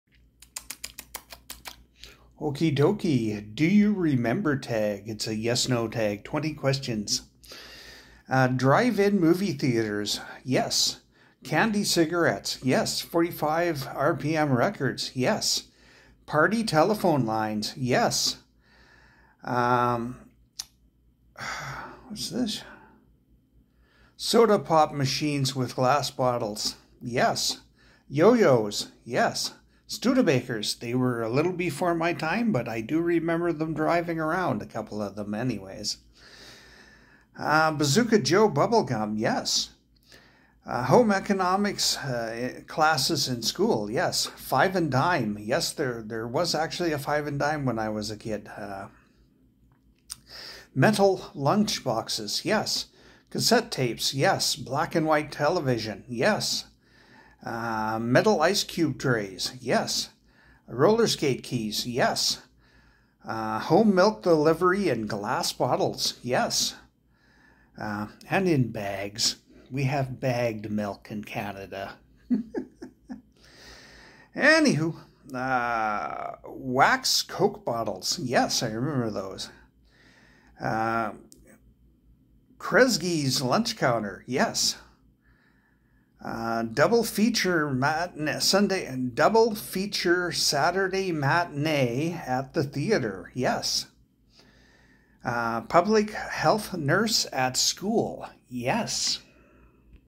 DO YOU REMEMBER TAG
1 Drive-in movie theaters?
2 Candy cigarettes?
3 45 rpm records?
4 Party telephone lines?
5 Soda pop machines with glass bottles?
6 Yo-Yo's?
7 Studabakers?
8 Bazooka Joe bubble gum?
9 Home economics classes in school?
10 5 and dime?
11 Metal lunchboxes?
12 Cassette tapes?
13 Black and white TVs?
14 Metal ice cube trays?
15 Roller skate keys?
16 Home milk delivery in glass bottles?
17 Wax Coke bottles?
18 Kresge's lunch counter?
19 Double feature Saturday Maintenance at the theater?
20 Public health nurse at school?